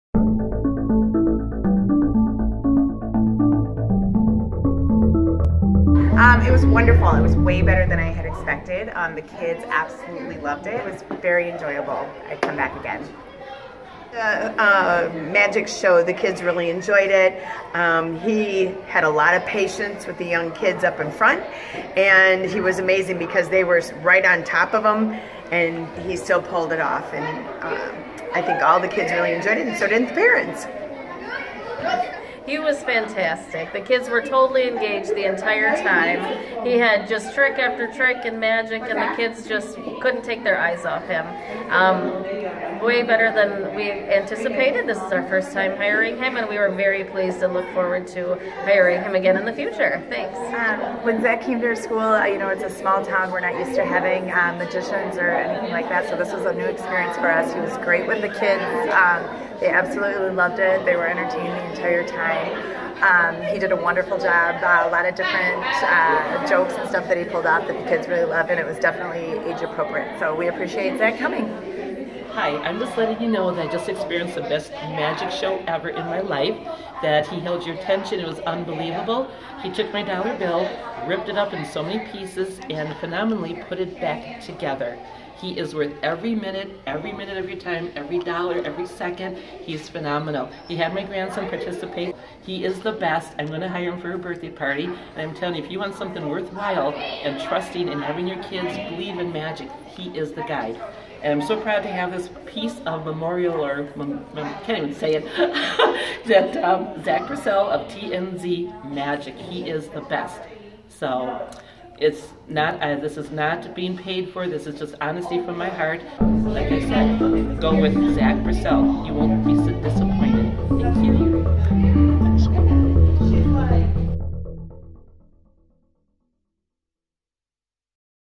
Testimonials Mother/Son Event
5 moms wanted to share their feelings about Zack's magic show at their mother/son banquet in Orfordville, Wisconsin.